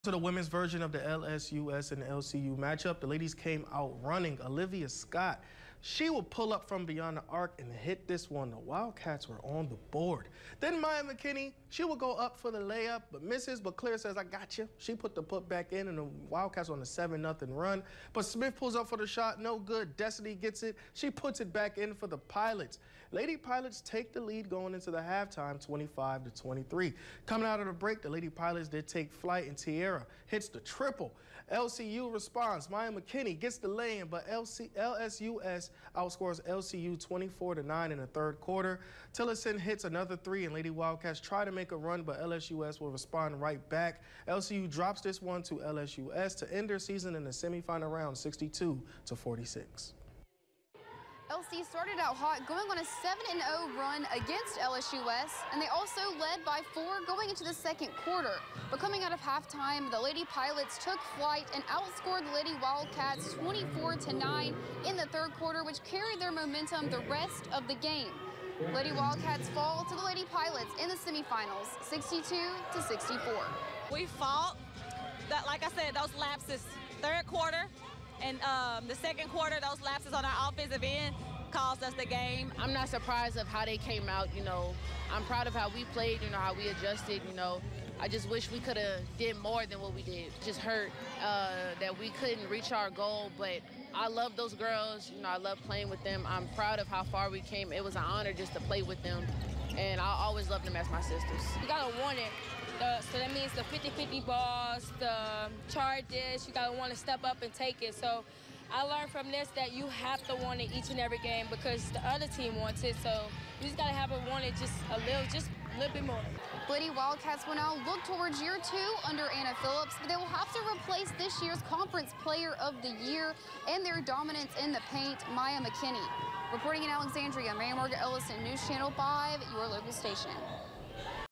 LCU Lady Wildcats fall to LSUS Lady Pilots in Semifinals of RRAC tournament
The Louisiana Christian Lady Wildcats faced the LSU-Shreveport Lady Pilots in the semifinal round of the RRAC tournament.